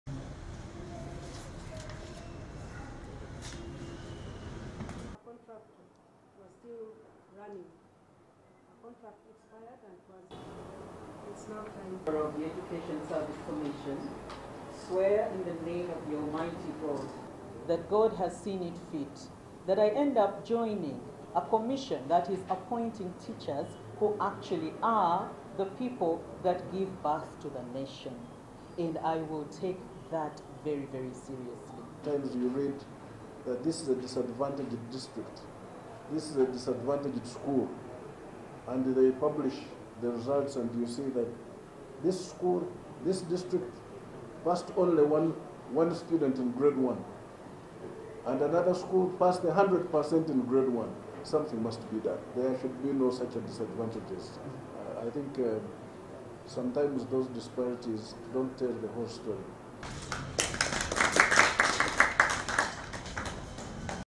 Education service commission gets new commissioners
The Chief Justice Bart Katurebe has raised concern over the glaring regional imbalances in the education sector in terms of quality. The Chief Justices made the remarks during the swearing in ceremony of Rose Kabogoza Musoke as a member of the Education Service Commission at the High Court in Kampala.